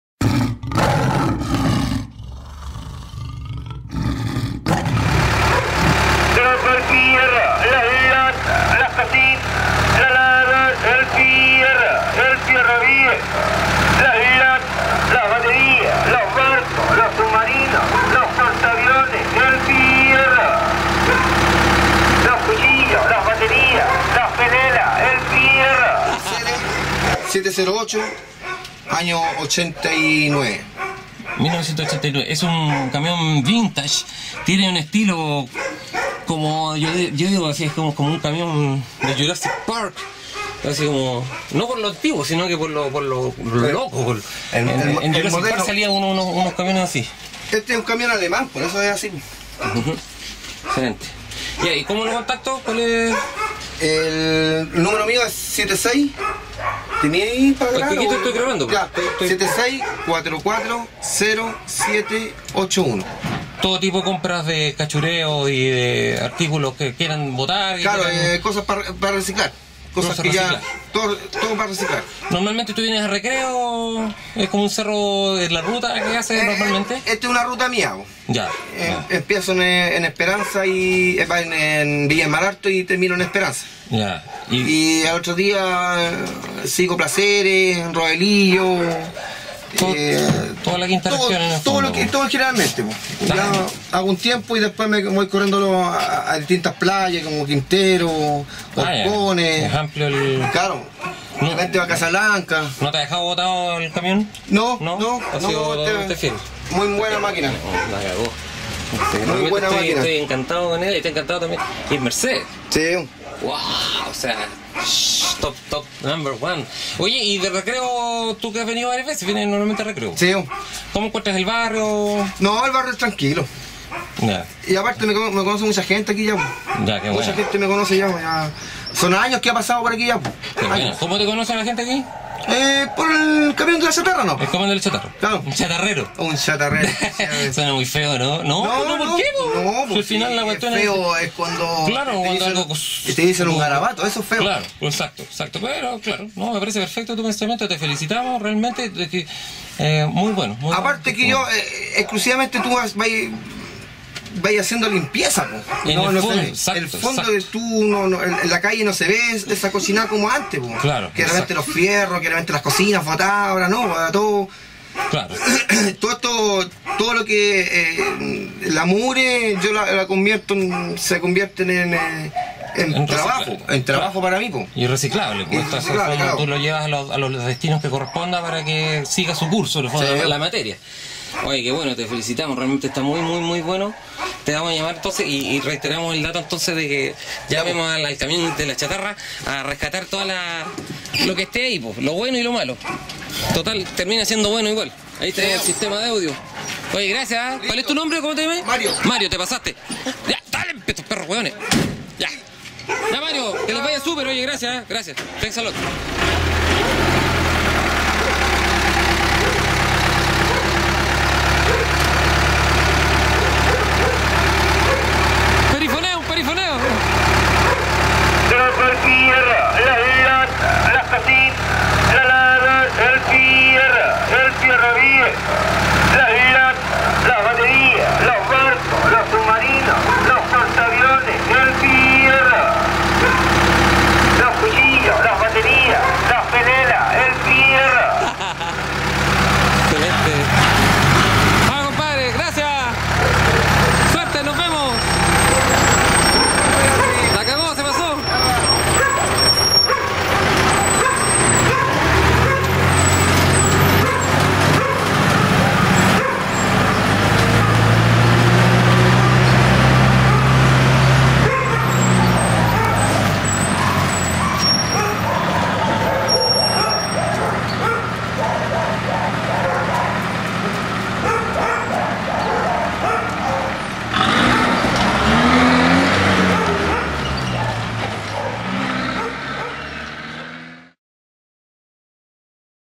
El Chatarrero de Recreo
Una nota con Don Mario el "Chatarrero" de Recreo.
Toda una vida entre fierros y chatarras.
Servicio de limpieza de chatarras gratuito.